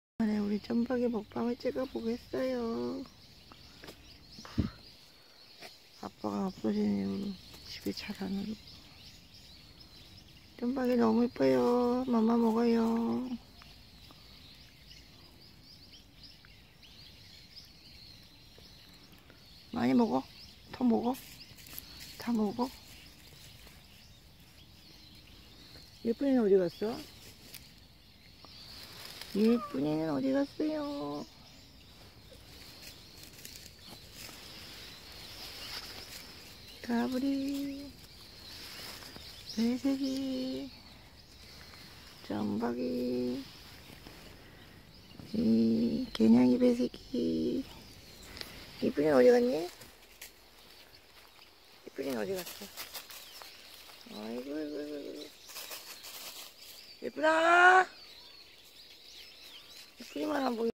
대장태비아들점박이와함께!//Black cat Tabby'son with Jermbakee!
대장태비부재이후,다시저한테밥먹으로오는데한달걸렸어요,아빠가없어서한달내내울더라고요,저만보면,아빠를찾아달라는것처럼,이부자는5년넘게마당냥이로지냈거든요,고양이도외로움을많이타요...!//After Tabby's absence it took a month his son come to eat,come to me, he had been crying for a month because  of his father ,as if  he wanted me to his bring his father again.
Ths father and 2 sons(the other'son is Noraengee)are lived in my yard over 5 years, as yard cats.they seem to feel lonely as people do!.